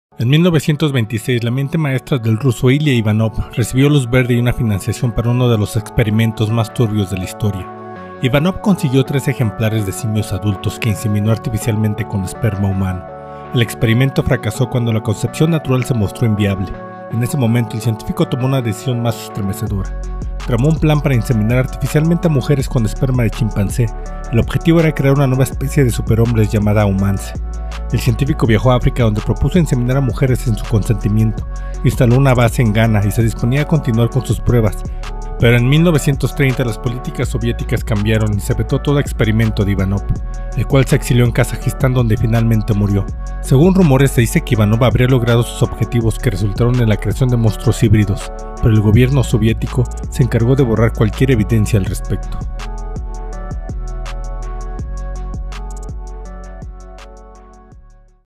El experimento más turbio de la historia
En 1926 la mente maestra de ruso Iliá Ivanov, recibió luz verde y una financiación para uno de los experimentos más turbios de la historia. Ivanov consiguió tres ejemplares de simios adultos que inseminó artificialmente con esperma humano. El experimento fracasó cuando la concepción natural se mostró inviable. En ese momento el científico tomo una decisión más estremecedora. Tramo un plan para inseminar artificialmente a mujeres con esperma de chimpancé. El objetivo era crear una nueva especie de superhombres llamada Humancé. El científico viajó a África donde propuso inseminar a mujeres sin su consentimiento. Instalo una base en Ghana y se disponía a continuar con sus pruebas, pero en 1930 las políticas soviéticas cambiaron y se vetó todo experimento de Ivanov, el cual se exilió en Kazajistan donde finalmente murió. Según rumores se dice que Ivanov habría logrado sus objetivos que resultaron en la creación de monstruos híbridos, pero el gobierno soviético se encargó de borrar cualquier evidencia al respecto.